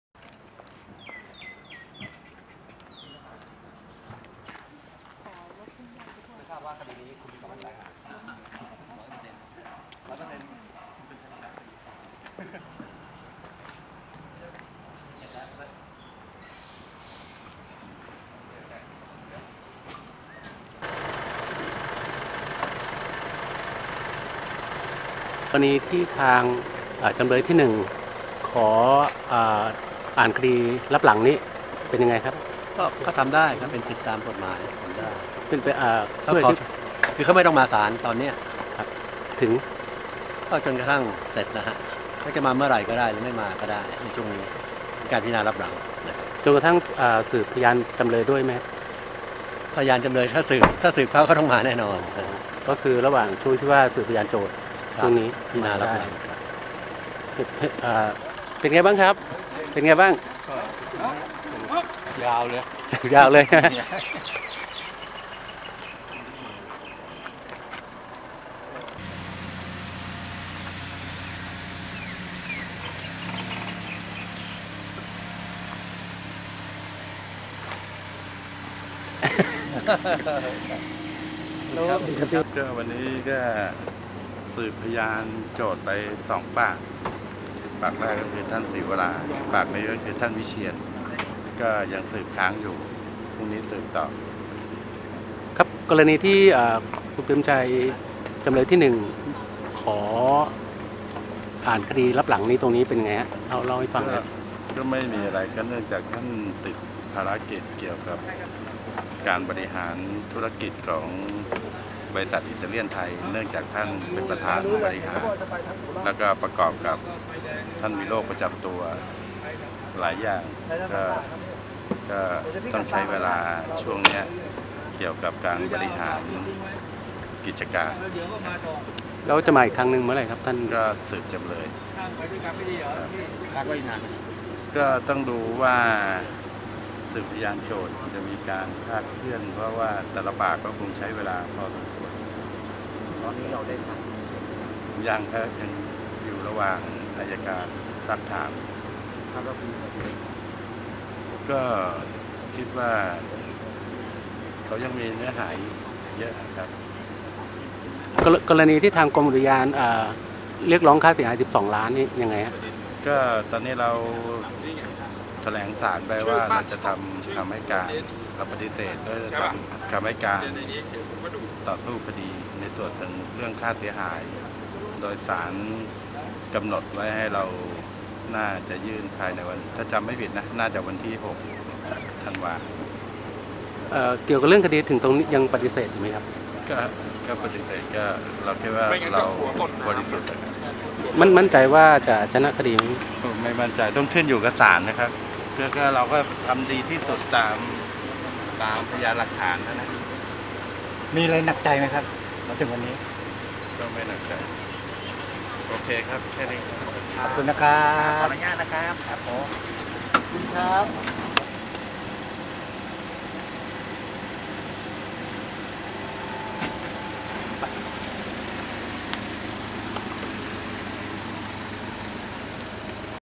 ทนาย "เปรมชัย" เผยไม่มั่นใจชนะคดี ขึ้นอยู่ที่ศาลจะตัดสิน พร้อมได้สิทธิไม่ต้องมาฟังการสืบพยานโจทก์
ทนาย "เปรมชัย" เผยไม่มั่นใจชนะคดี ขึ้นอยู่ที่ศาลจะตัดสิน พร้อมได้สิทธิไม่ต้องมาฟังการสืบพยานโจทก์ 27/11/2018